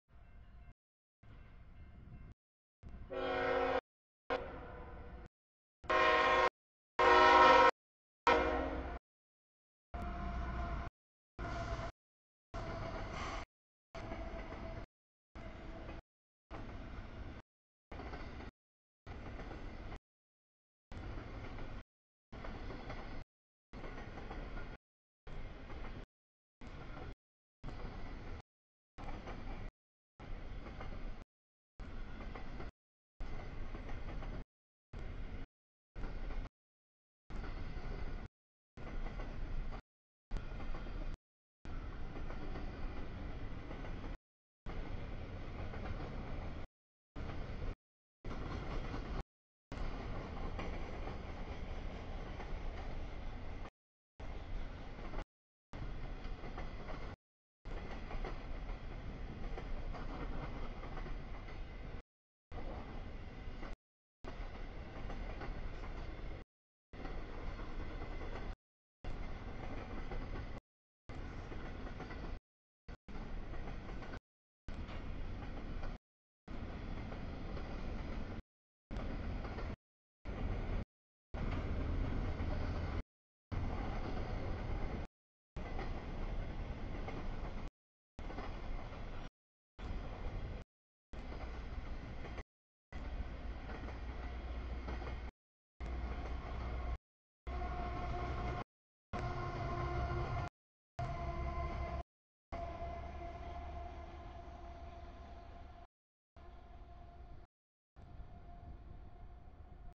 3- 11-2019 01:18 nb stack CSX Q157
00:25 nb stack CSX 295 Q159
00:39 nb stack trailers CSX 53 Q009
01:18 nb stack CSX Q157
02:07 sb stack CSX 3038 Q162
03:23 sb stack CSX Q160
03:58 nb auto rack CSX Q261
06:26 sb stack trailers CSX 967 Q010
07:13 sb stack CSX 5291, 3212, 3125 Q158
07:36 sb stack trailers CSX 3010, 3276 Q008
07:40 nb stack trailers CSX 3033, 3159 L003
08:48 sb mix CSX 3031, 4060 DPU 3299 Q409
09:22 nb auto rack CSX 91 UP 8684 Q253
11:58 nb empty's BNSF 4721, 5537 K143